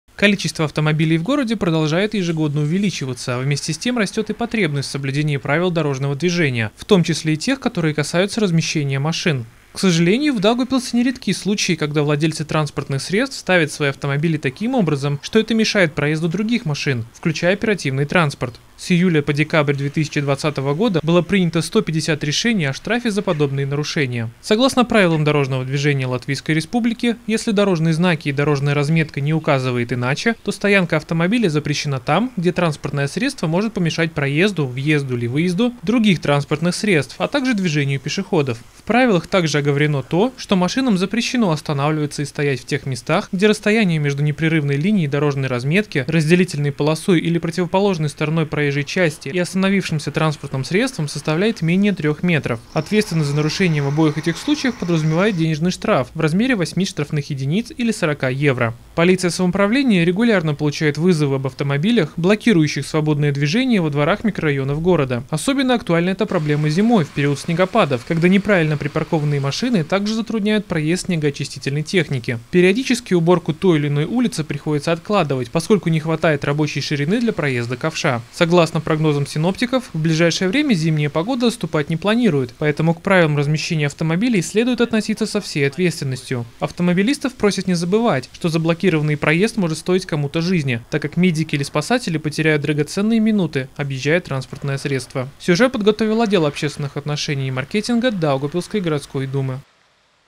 Водителей призывают не блокировать улицы и подъездные пути
Количество автомобилей в городе продолжает увеличиваться, а вместе с тем растет и потребность в соблюдении правил дорожного движения, в том числе и тех, которые касаются размещения машин. К сожалению, в Даугавпилсе не редки случаи, когда владельцы транспортных средств ставят свои автомобили таким образом, что это мешает проезду других машин, включая оперативный транспорт (скорая помощь, пожарная бригада). С июля по декабрь 2020 года было принято 150 решений о штрафе за подобные нарушения.